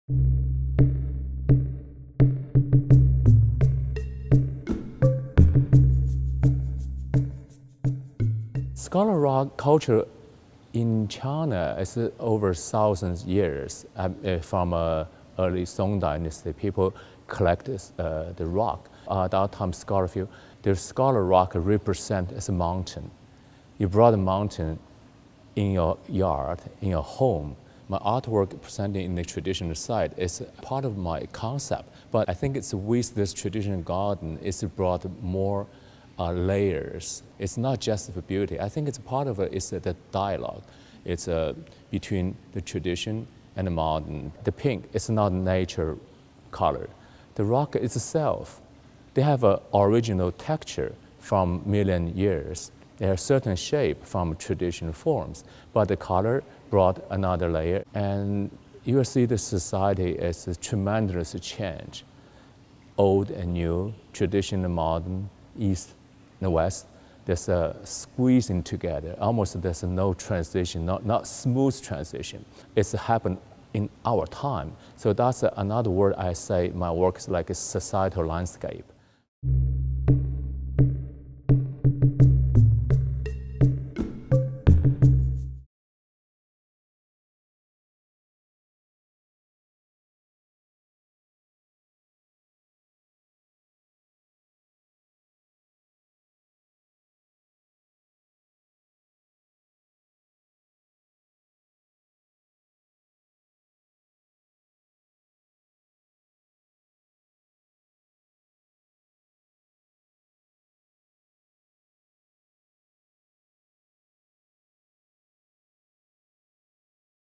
Zhang Jianjun Discusses His Work in "Ink Art: Past as Present in Contemporary China"
Zhang Jianjun discusses his work "Scholar Rock" (2008) as part of the artist interview series conducted for the special exhibition "Ink Art: Past as Present in Contemporary China."

Credits

Producers: Chris Noey and Howard Silver

Camera Operators: Jessica Glass and Scott Sinkler

Interviewer: Xin Wang

Editor: Marina Zarya

Scholarly Consultants: Maxwell K. Hearn and Xin Wang